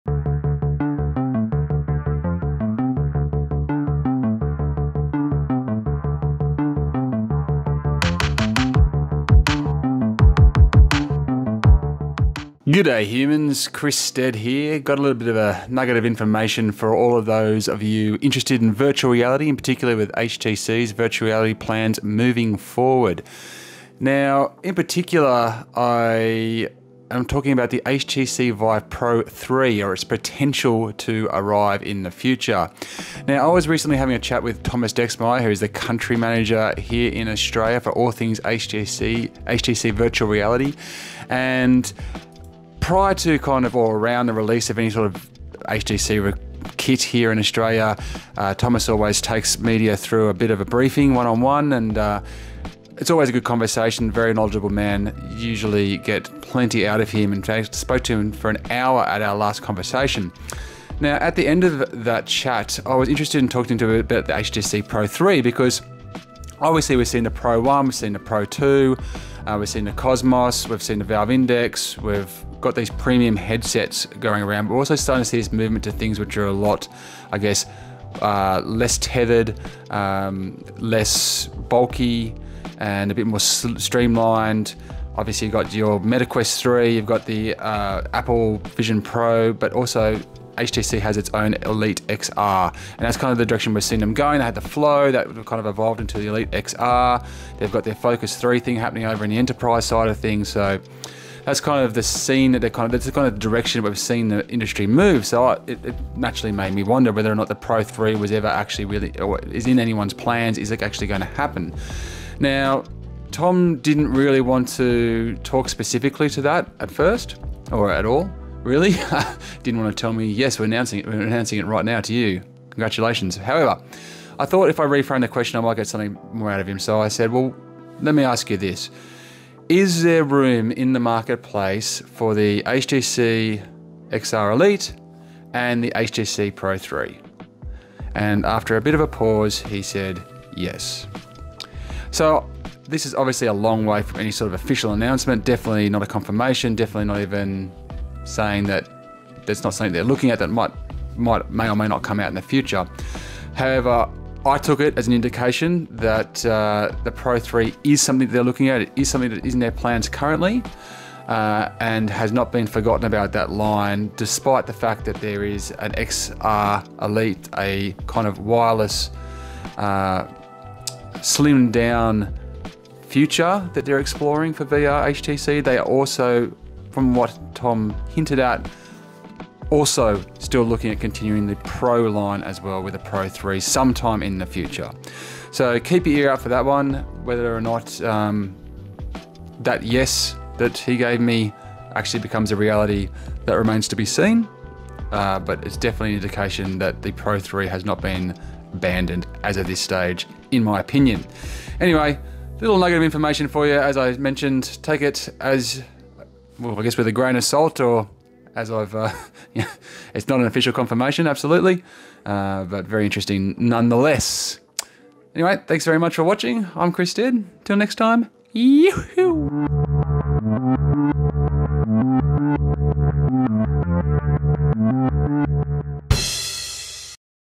Confirmed: HTC Vive Pro 3 VR headset is still possible
After the successful release of the premium HTC Vive Pro and HTC VIve Pro 2 VR headsets, as well as the enterprise focused HTC Vive Focus 3, the company seemed to be going in a different direction. The HTC Vive Flow, which was then followed by the HTC Vive XR Elite, both veer away from the large PCVR headset design into a wireless, mixed reality, smart glasses like approach. Does that mean that the virtual reality pioneers won’t be releasing an HTC Vive Pro 3?

Technology journalist Chris Stead recently spoke to HTC and directly asked the question about the possibility of the HTC Vive Pro 3 rumors being true. In this video, he discusses the company’s response. There is no HTC Vive Pro 3 release date as yet.

NIK2SGVB2GGO8YPA